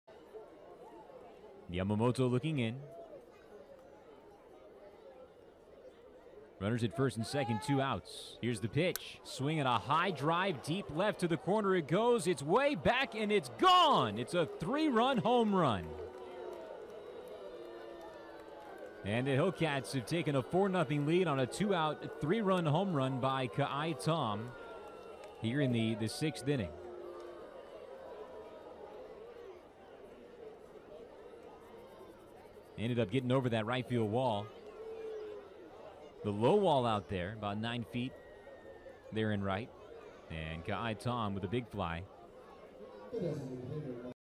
Tom crushes three-run blast for Lynchburg
8/10/17: Ka'ai Tom crushes a three-run blast in the sixth inning of the Hillcats' 5-0 win against the Mudcats